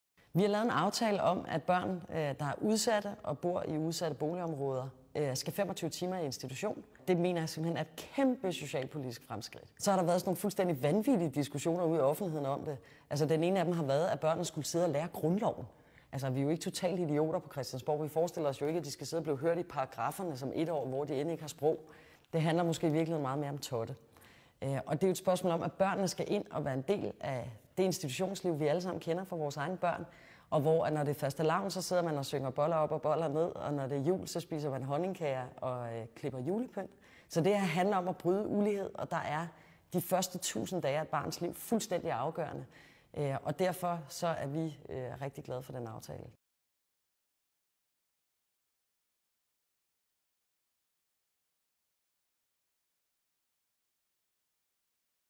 Vi vil bekæmpe ulighed i samfundet
Vi har indgået en aftale, der skal bekæmpe parallelsamfund. Og vi tør godt sige, at den er et stort socialpolitisk fremskridt.
For det handler ikke om at småbørn skal terpe grundloven. Den handler om at bekæmpe ulighed i samfundet.
Med denne aftale kan vi forhåbentlig ændre udsatte børns livsbane og give dem bedre muligheder for et godt liv 🌹